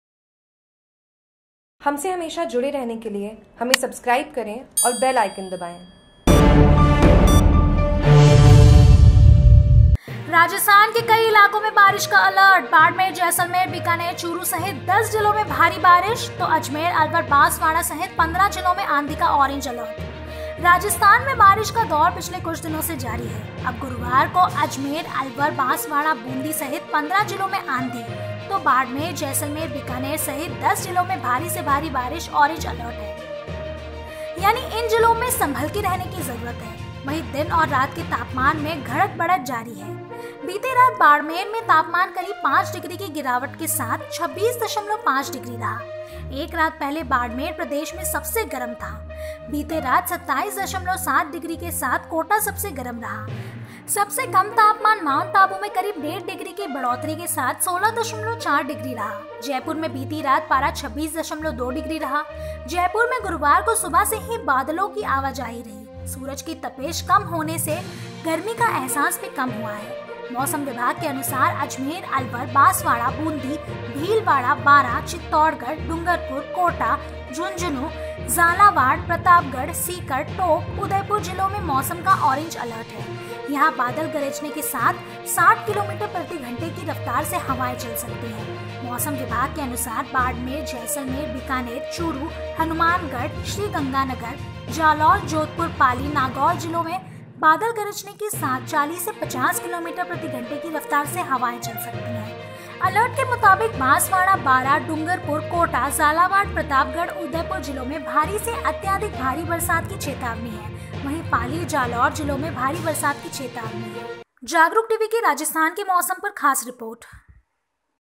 Jagruk TV : राजस्थान के कई इलाकों में बारिश का अलर्ट
बाड़मेर, जैसलमेर, बीकानेर, चूरू सहित 10 जिलों में भारी बारिश तो अजमेर, अलवर, बांसवाड़ा सहित 15 जिलों में आंधी का ऑरेंज अलर्ट

राजस्थान में बारिश का दौर पिछले कुछ दिनों से जारी है। अब गुरुवार को अजमेर, अलवर, बांसवाड़ा, बूंदी सहित 15 जिलों में आंधी तो बाड़मेर, जैसलमेर, बीकानेर सहित 10 जिलों में भारी से भारी बारिश ऑरेंज अलर्ट है। यानी इन जिलों में संभलके रहने की जरूरत है। वहीं दिन और रात के तापमान में घटत-बढ़त जारी है।

बीती रात बाड़मेर में तापमान करीब पांच डिग्री की गिरावट के साथ 26.5 डिग्री रहा। एक रात पहले बाड़मेर प्रदेश में सबसे गर्म था। बीती रात 27.7 डिग्री के साथ कोटा सबसे गर्म रहा। सबसे कम तापमान माउंट आबू में करीब डेढ़ डिग्री की बढ़ोतरी के साथ 16.4 डिग्री रहा। जयपुर में बीती रात पारा 26.2 डिग्री रहा। जयपुर में गुरुवार को सुबह से ही बादलों की आवाजाही रही। सूरज की तपिश कम होरे से गर्मी का अहसास भी कम हुआ है।

मौसम विभाग के अनुसार अजमेर, अलवर, बांसवाड़ा, बूंदी, भीलवाड़ा, बारां, चित्तौड़गढ़, डूंगरपुर, कोटा, झुंझुनूं, झालावाड़, प्रतापगढ़, सीकर, टोंक, उदयपुर, जिलों में मौसम का ऑरेंज अलर्ट है। यहां बादल गरजने के साथ 60 किमी प्रतिघंटे की रफ्तार से हवाएं चल सकती हैं।

मौसम विभाग के अनुसार बाड़मेर, जैसलमेर, बीकानेर, चूरू, हनुमानगढ़, श्रीगंगानगर, जालौर, जोधपुर, पाली, नागौर जिलों में बादल गरजने के साथ 40 से 50 किमी प्रति घंटा की रफ्तार से हवाएं चल सकती हैं।

अलर्ट के मुताबिक बांसवाड़ा, बारां, डूंगरपुर, कोटा, झालावाड़, प्रतापगढ़, उदयपुर जिलों में भारी से अत्यधिक भारी बरसात की चेतावनी है। वहीं पाली, जालौर, जिलों में भारी बरसात की चेतावनी है।

Jagruk TV